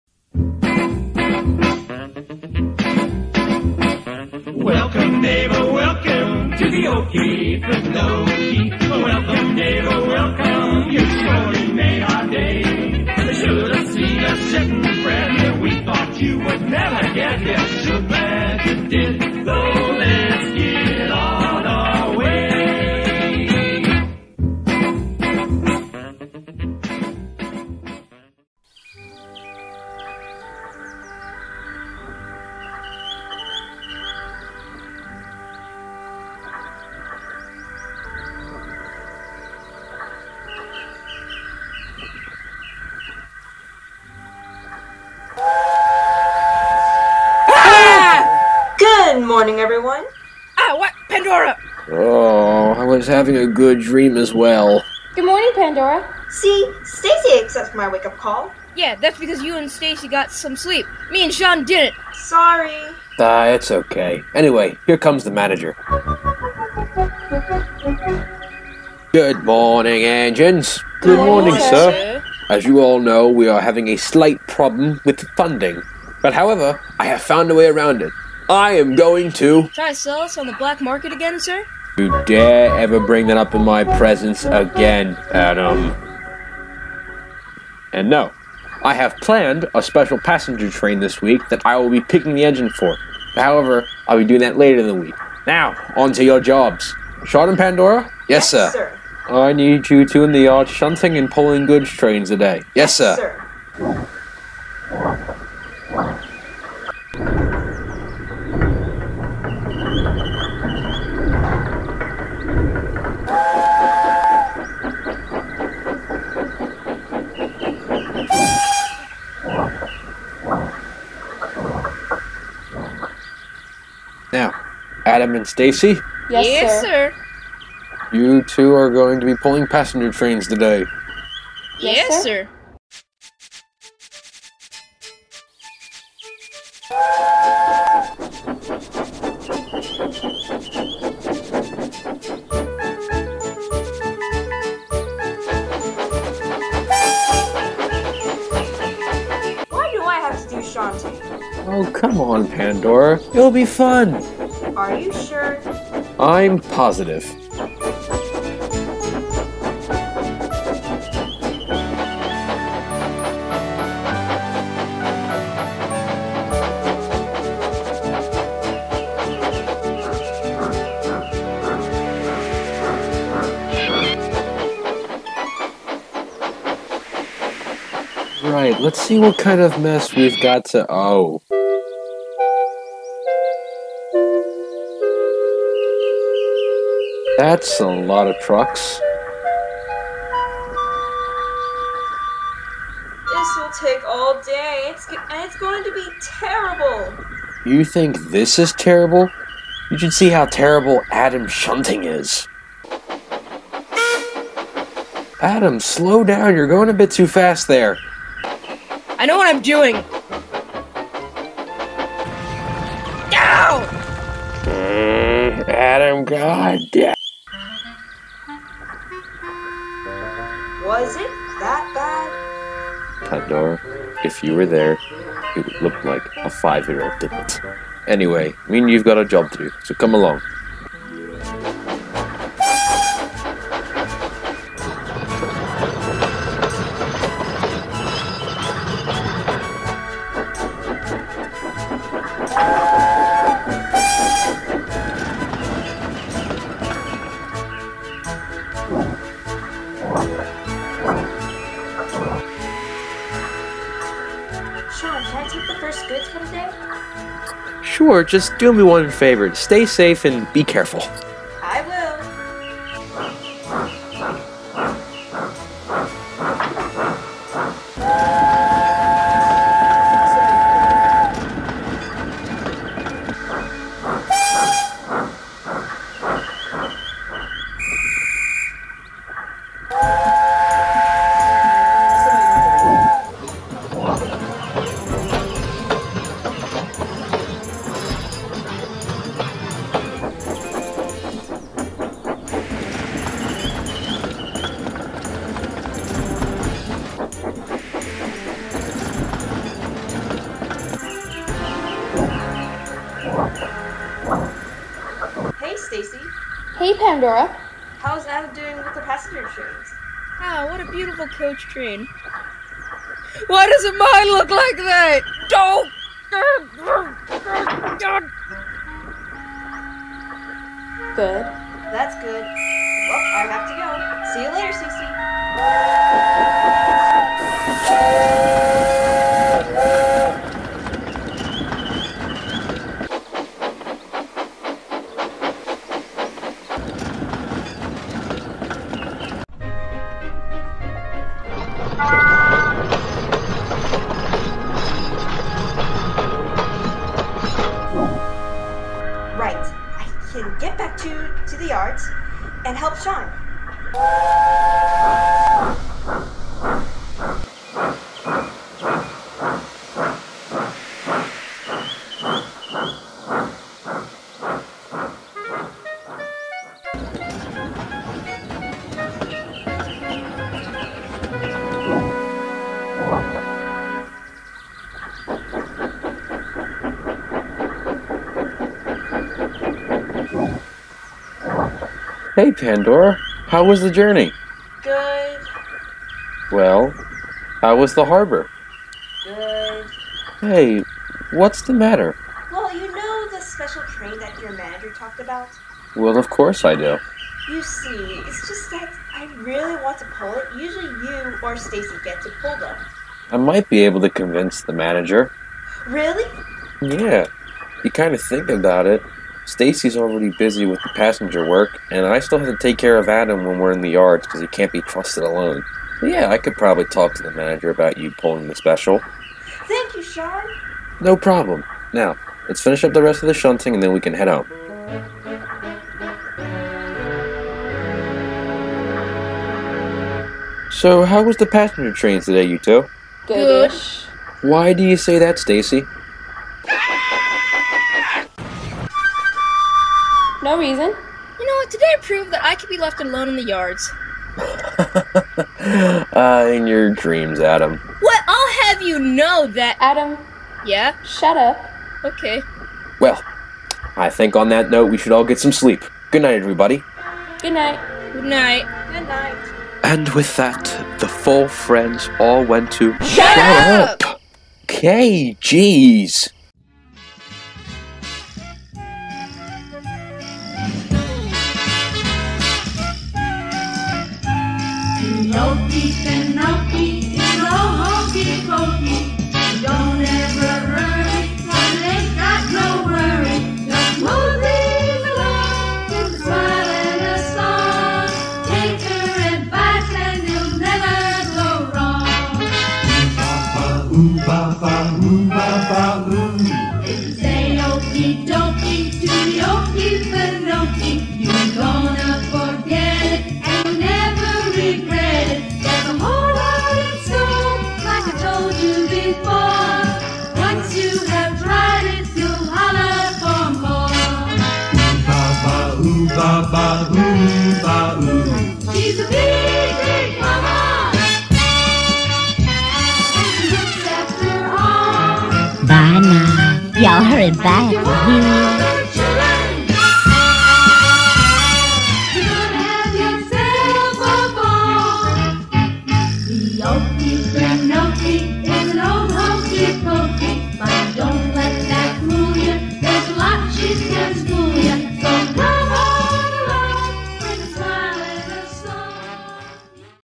The Okefenokee Railway Ep.1: The Pilot
Hey guys. here is episode 1 of the new series me and my friend AdamCat are making I hope you all enjoy

Cast
Sean    Me
Adam    AdamCat
Pandora   Gremlin
Stacy      KYLE61
Manager   Me
Narrator  Me

Music
Carson080220
Tim Hollis
S.A Music
Mavis M

Sound effects
G.W Shunter
Terrier55stepney

Ditties
Carson080220
Skarloey Rheneas
TheGermanofSodor